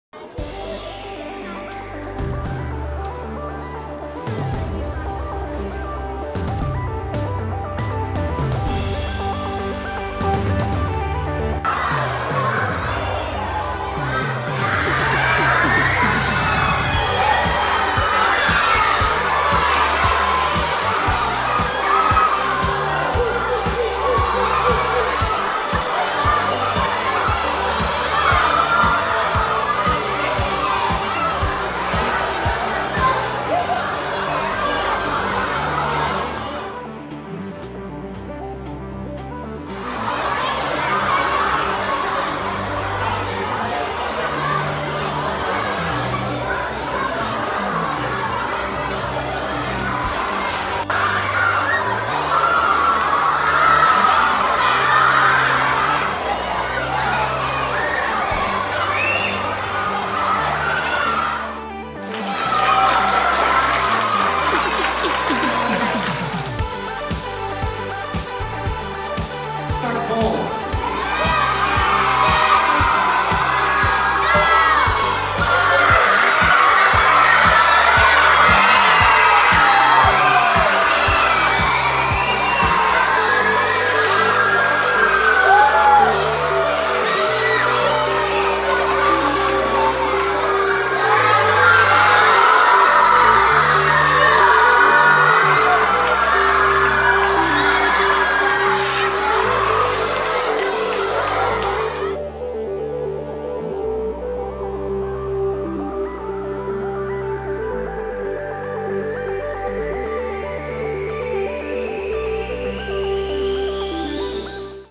One Book, One School - Closing Assembly
As we conclude March is Reading month, we celebrated Coloma Elementary's completion of Mysteries According to Humphrey with special guest, Humphrey!